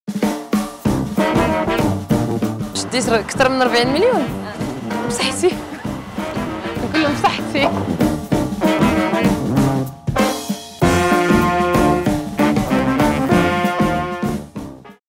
ليلى الحديوي تجيب عبر ‫#‏سلطانة‬ على الإشاعات
ليلى الحديوي تجيب عبر ‫#‏سلطانة‬ على من يروجون تقاضيها لأزيد من 40 مليون عن أحد الإشهارات،

تابعينا على soltana.ma لتكوني أنت السلطانة،